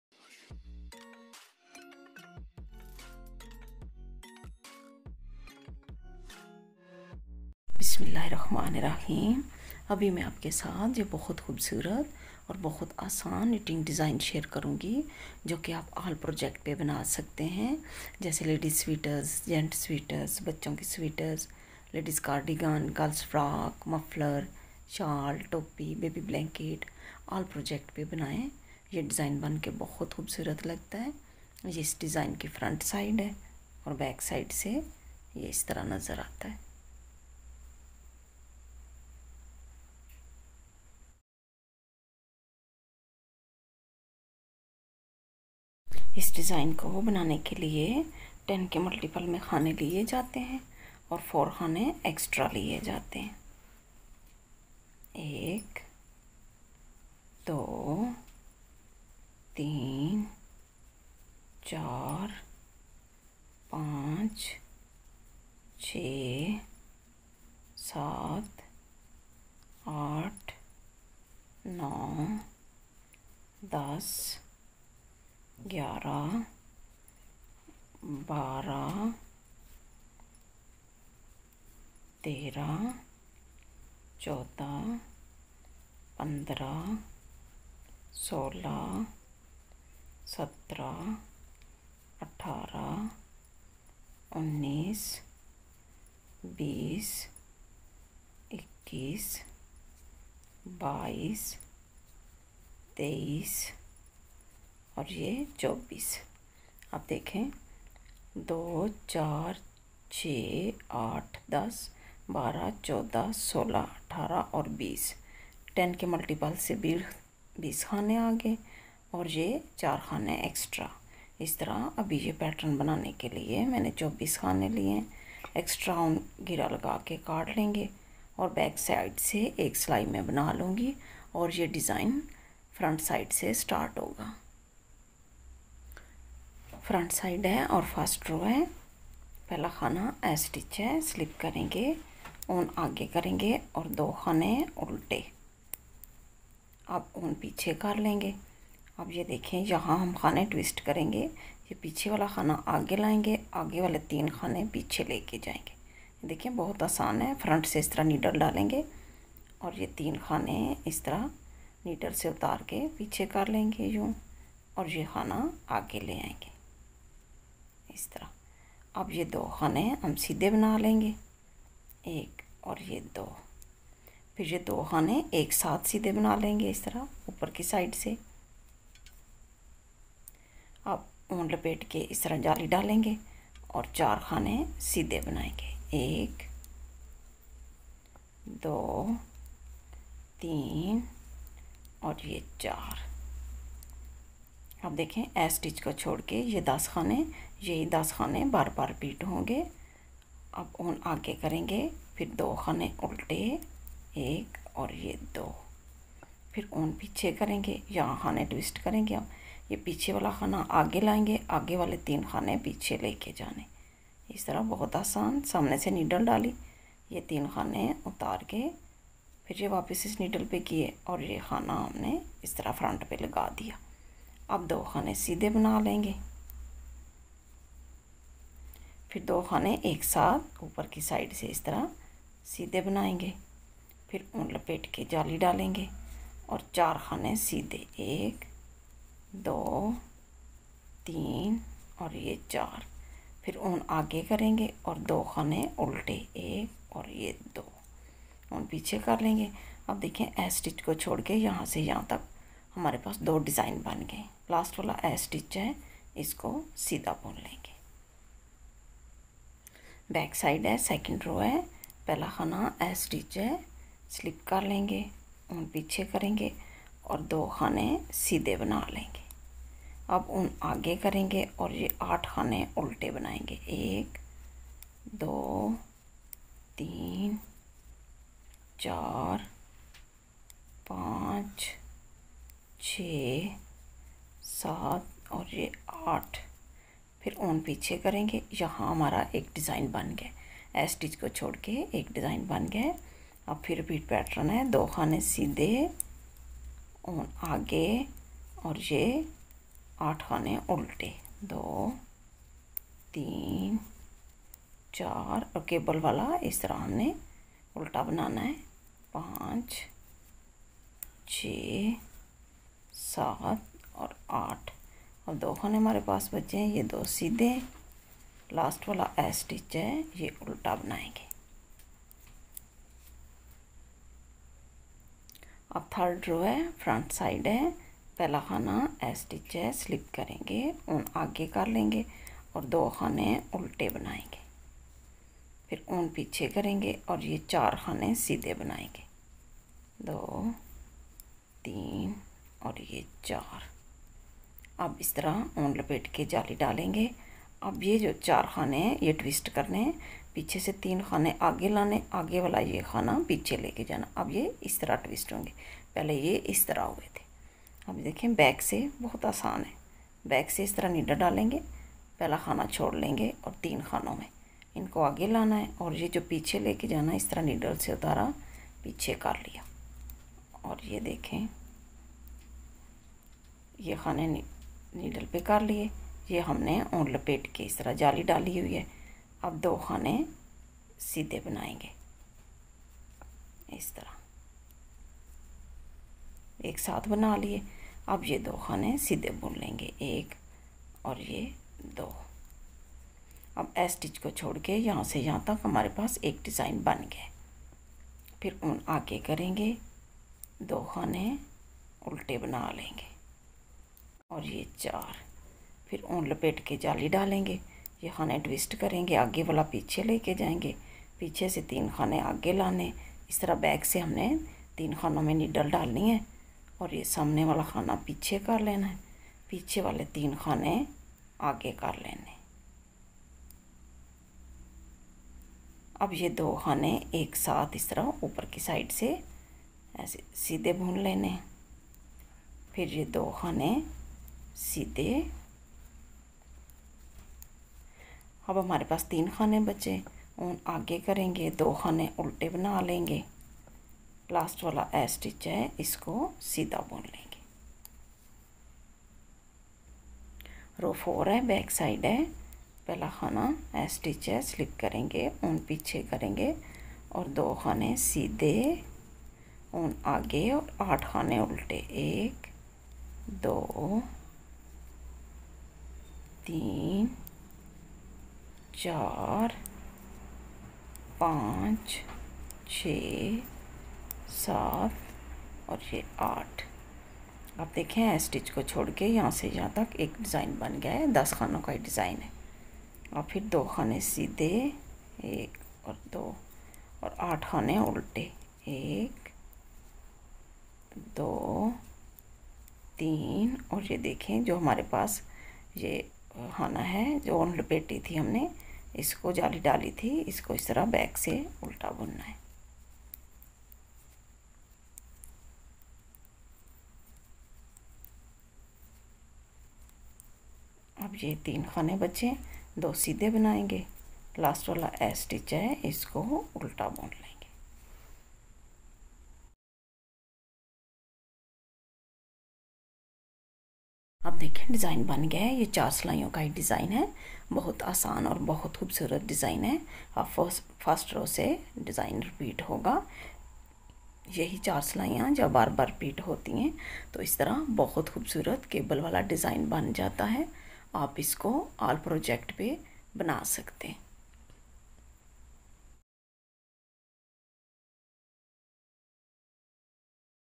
KNITTING DESIGN FOR LADEIS GENTS SWEETER GIRLS FROCK MUFFLER SHALLS CAP BACHON KI SWEETER JACKET
IN THIS VIDEO I WILL SHOW YOU THE KNIITING DEISIGN FOR  LADEIS  GENTS SWEETER GIRLS FROCK MUFFLER SHALLS CAP BACHON KI SWEETER JACKET F KNITTING DESIGNS